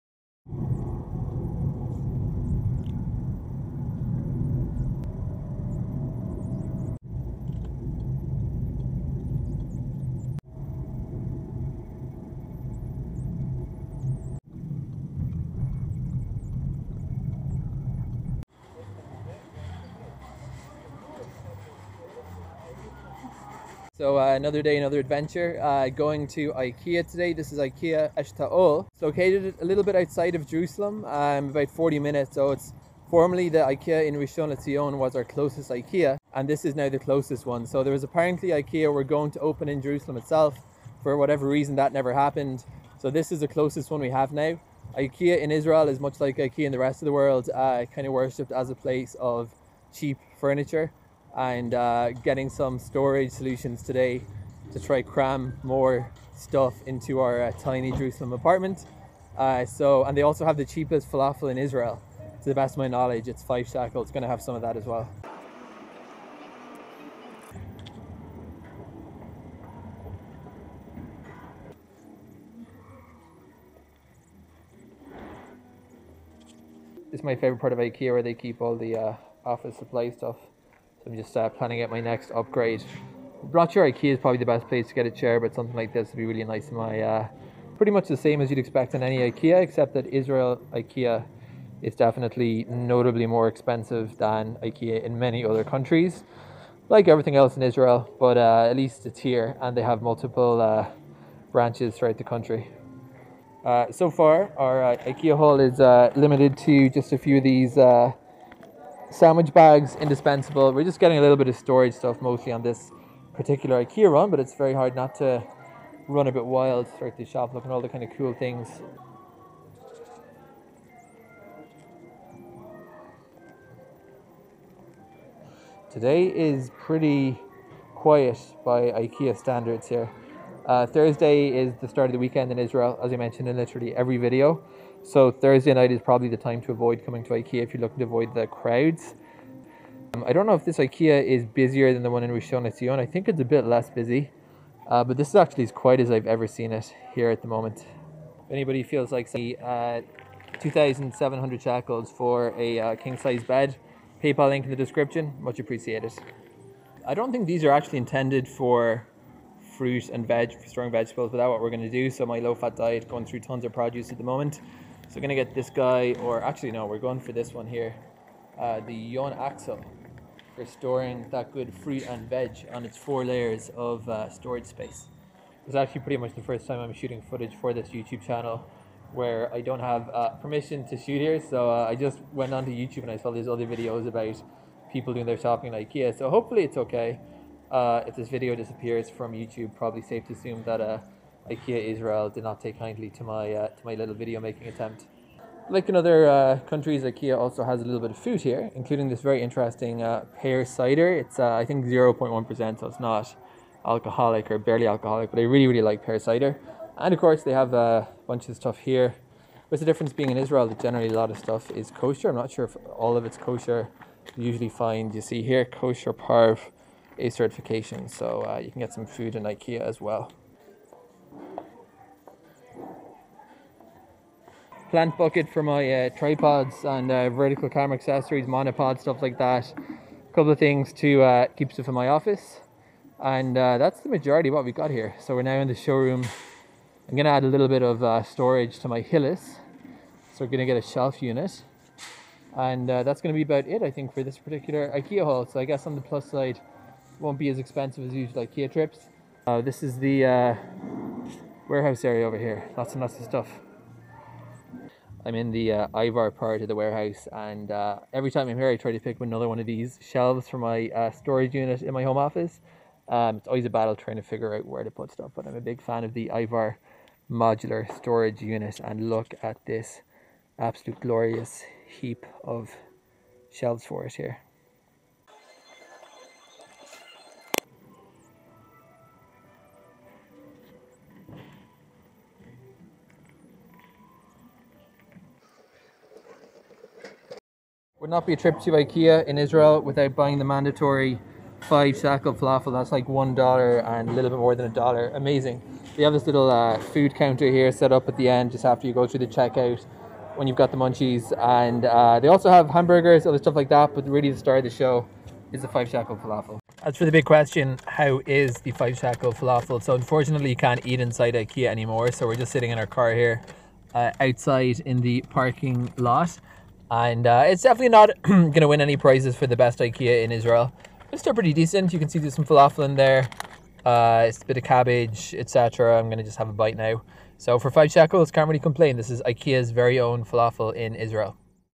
Checking Out Jerusalem's Closest IKEA At Eshtaol, Israel
I stopped by IKEA Esthaol today which is now the closest IKEA to Jerusalem. IKEA was apparently in talks with the Jerusalem authorities to put an IKEA within the actual city but that never ended up happening. Instead, IKEA is a drive to the West of the city near the Eshtaol kibbutz. When in any IKEA in Israel, it's a must to indulge in some of the 5 NIS falafel - potentially the cheapest falafel in the country. As successful and stress-free as any trip I've had to IKEA and I now have a bunch more storage space!

== Production notes ==*

Camera: Fimi Palm 2
Audio: Internal
Stabilization:
Lighting:
Postproduction:
Other notes: